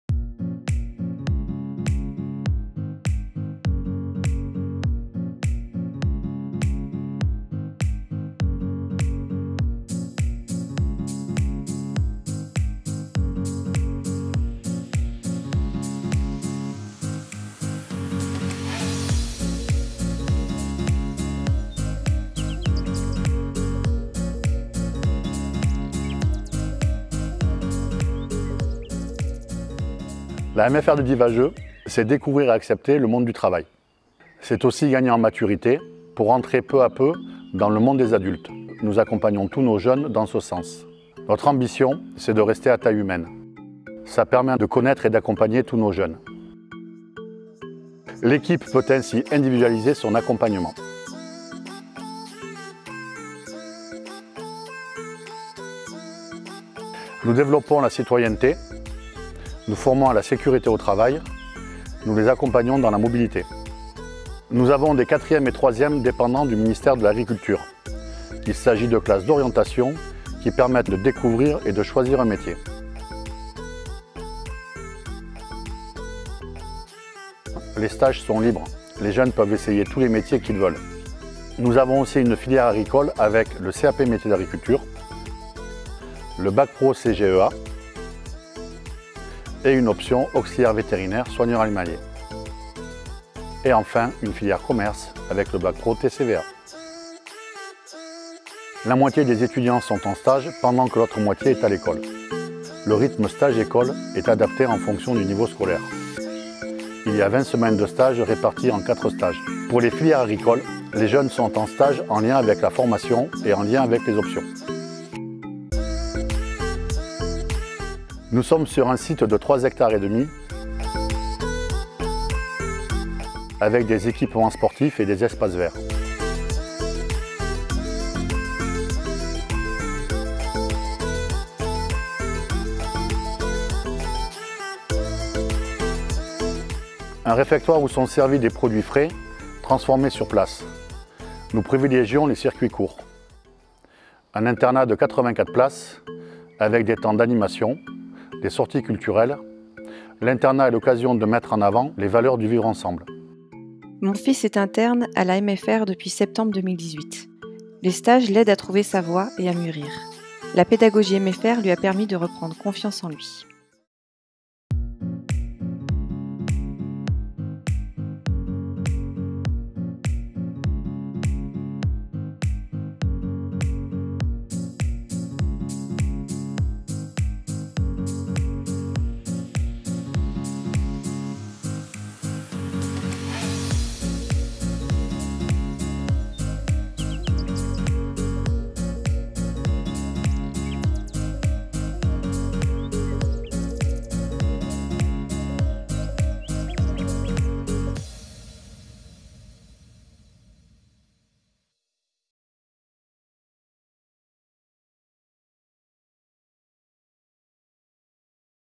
MFR Divajeu Présentation
Découvrez la MFR de Divajeu, dans la Drôme, près de Crest.
Classes d'orientation 4e et 3e, CAP Métiers d'agriculture, BAC Pro CGEA et TCVA.
Pour en savoir plus, rendez-vous sur notre ou sur notre page facebook MFR Divajeu